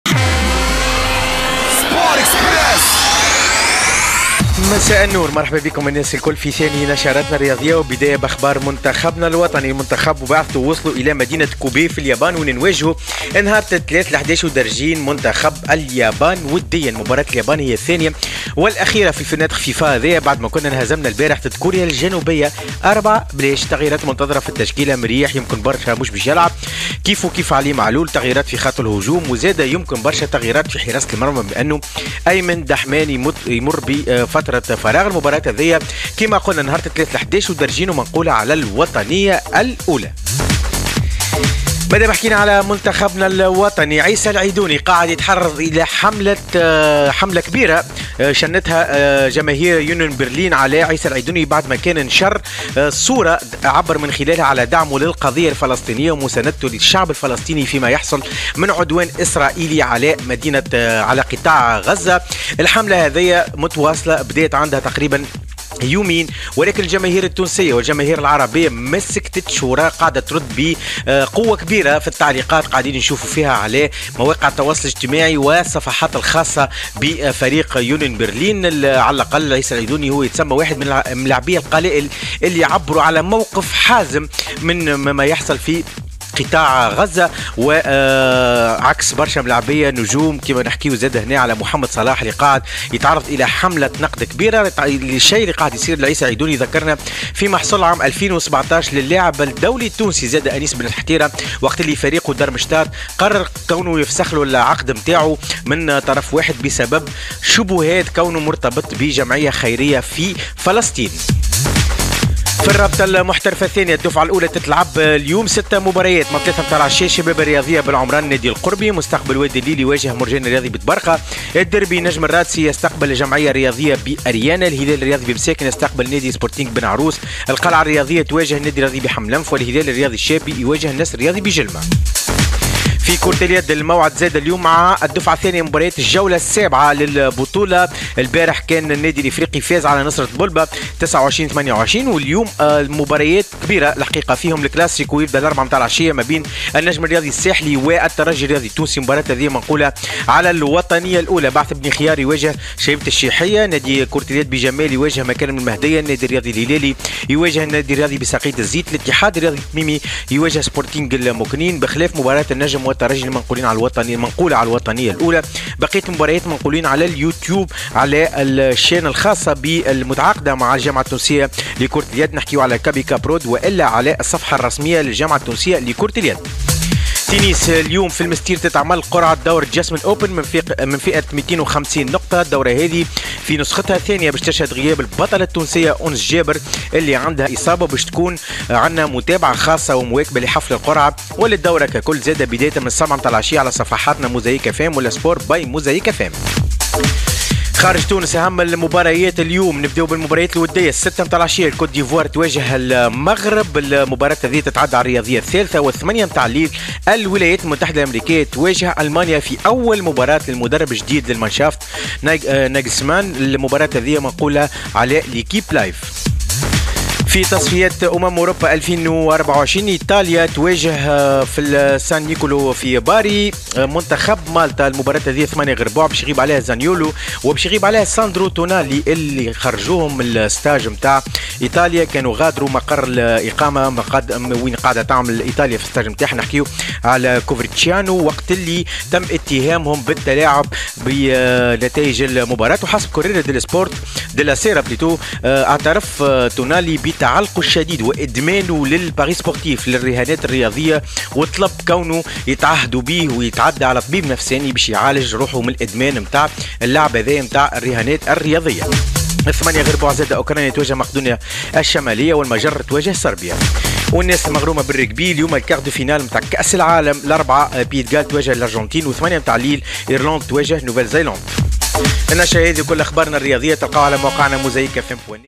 Sport Express : بعد بن حتيرة.. حملة ضد العيدوني والجماهير التونسية تعكس الهجوم
- بعد بن حتيرة.. حملة ضد العيدوني والجماهير التونسية تعكس الهجوم
- كلاسيكو جديد بين النجم والترجي
- تونالي يعترف بالإدمان ويطلب المساعدة
إشترك الأن على قناة اليوتوب